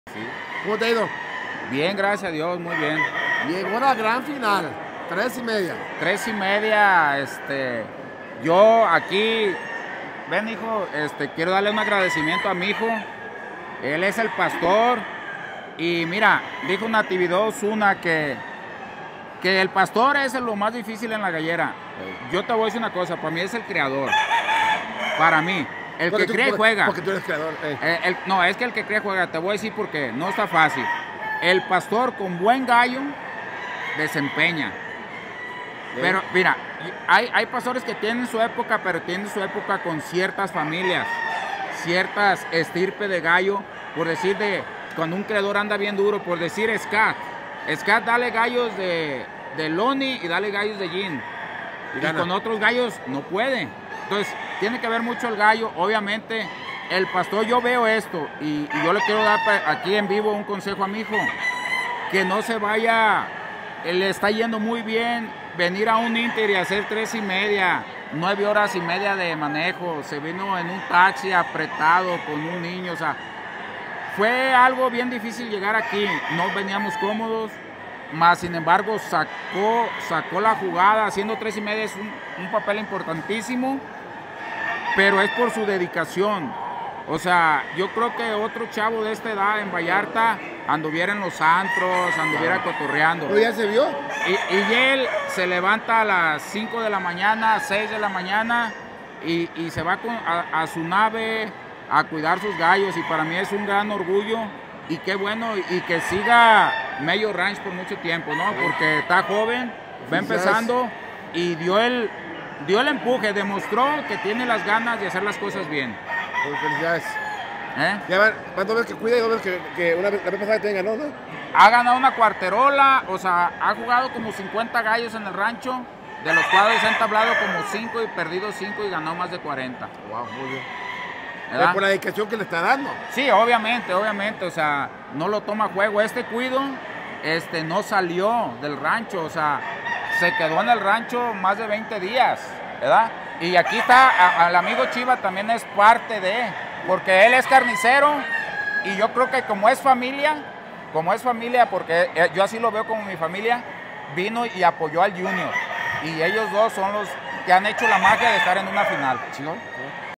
Mello en la Final
Mello nos presenta a su pastor y nos habla de su llegada a la final y que factores influyeron.